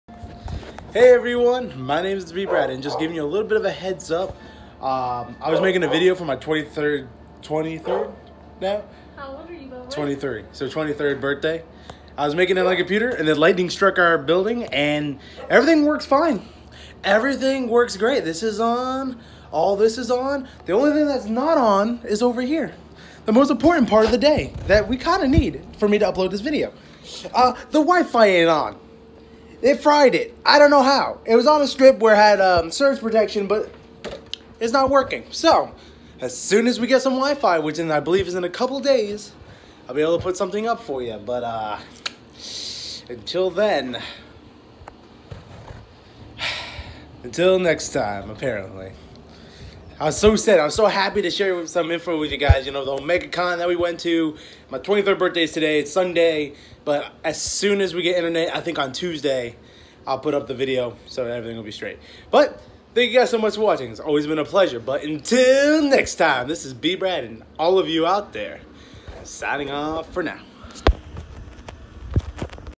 The outage update
Just an update why there will be no videos for 2 days